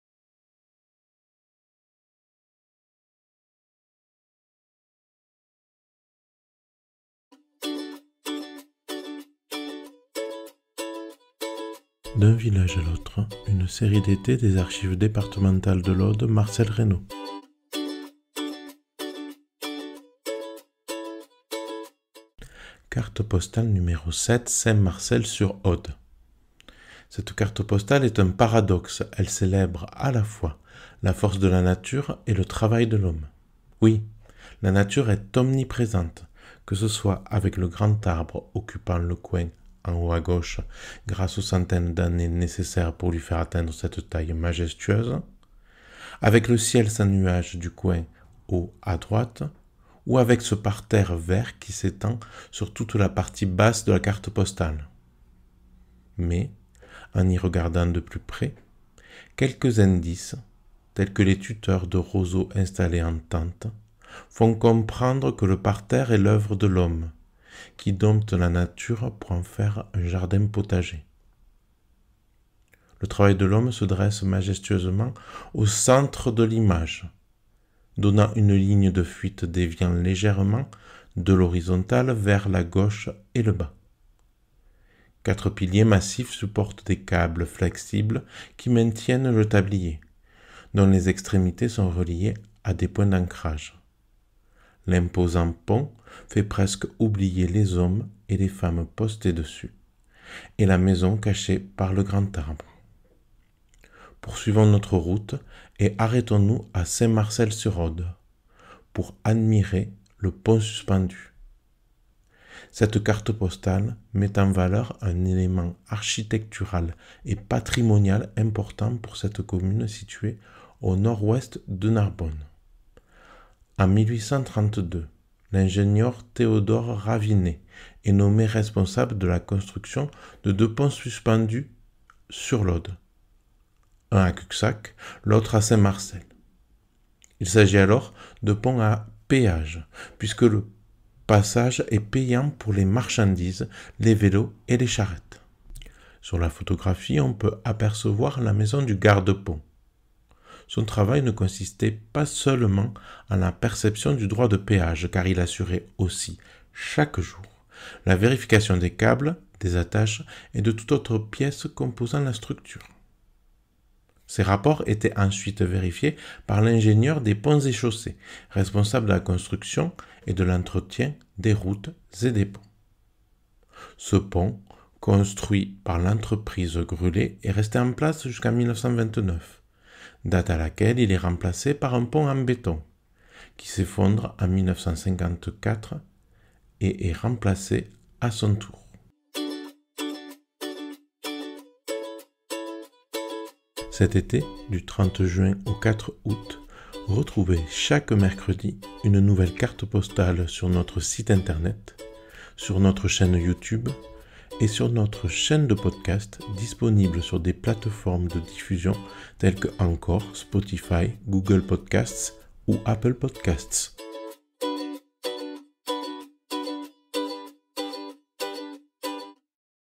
D'un village à l'autre #7 Saint-Marcel-sur-Aude
Poursuivons notre route et arrêtons-nous à Saint-Marcel-sur-Aude pour y admirer le pont suspendu.

Cette chronique vous est présentée dans la rubrique D'un village à l'autre , retrouvez-la également en Audio